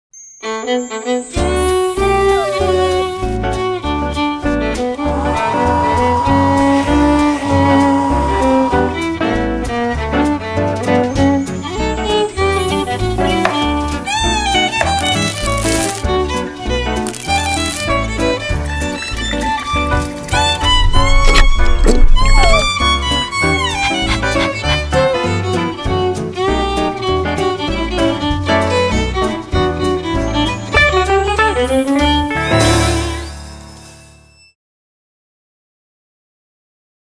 【食品類】Doggy Willie | 企業形象影片 | PIAD拍廣告
【PIAD拍廣告】20年豐富經驗的影片製作團隊，掌握影音行銷脈動

【Doggy Willie】
細心照顧，我們所愛的牠，就從每一餐開始
狗狗搭配小孩，不僅可愛，也表達著狗狗的飲食跟小孩一樣重要
可愛與小幽默的風格，令人為之融化